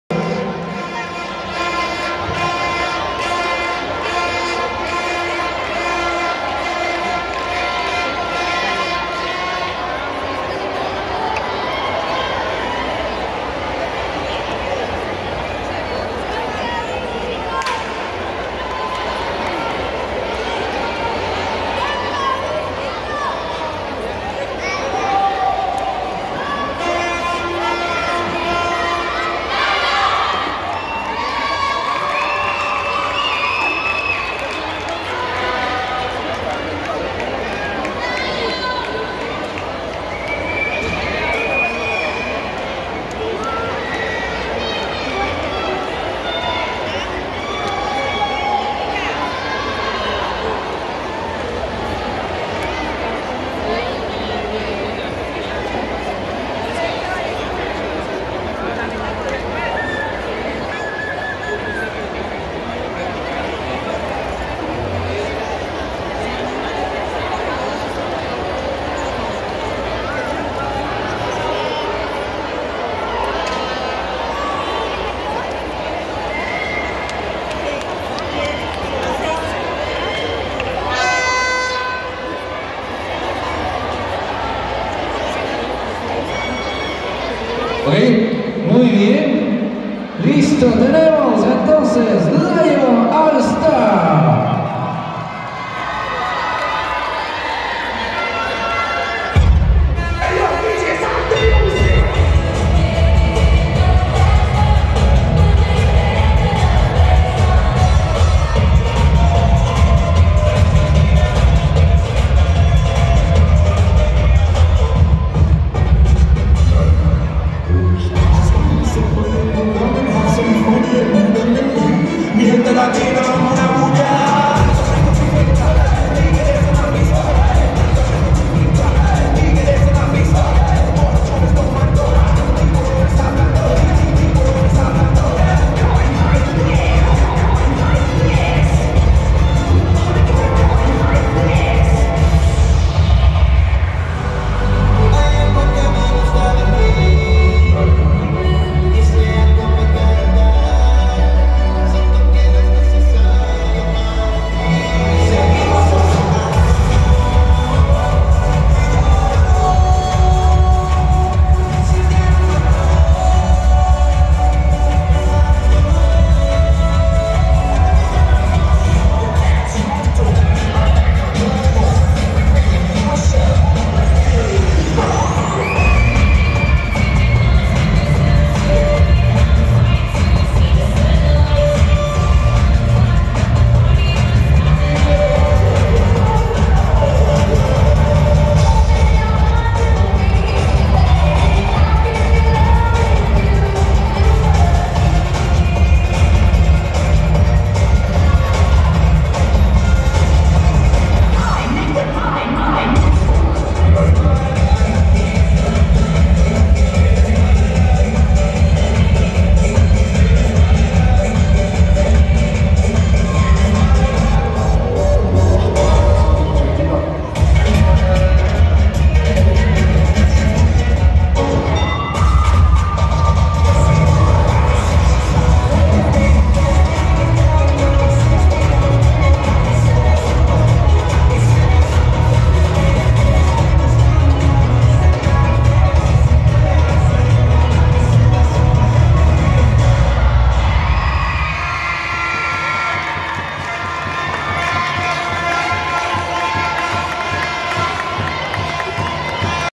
Grupo Lion's all stars en el campeonato Xtreme open Cheer & Dance
Equipo de porrismo Lion's all stars nivel 1 en el campeonato Panamericano Xtreme Cheer & Dance